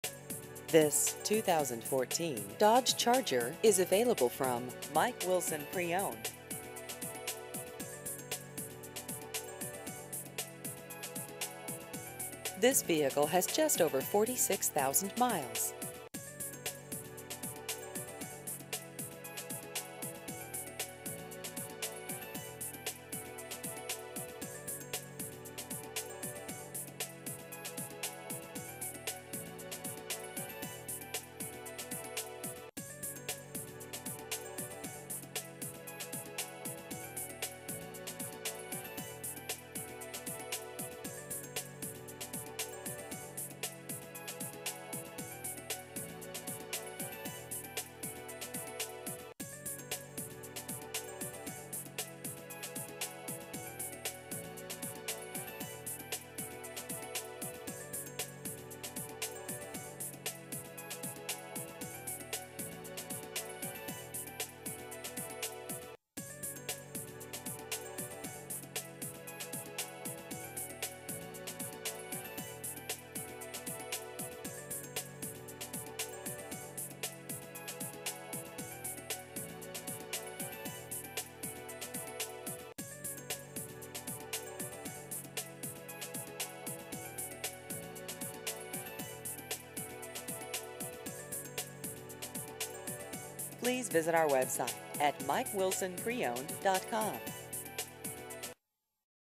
2014 Dodge Charger Used Cars Winchester KY
This 2014 Dodge Charger is available from Mike Wilson Preowned.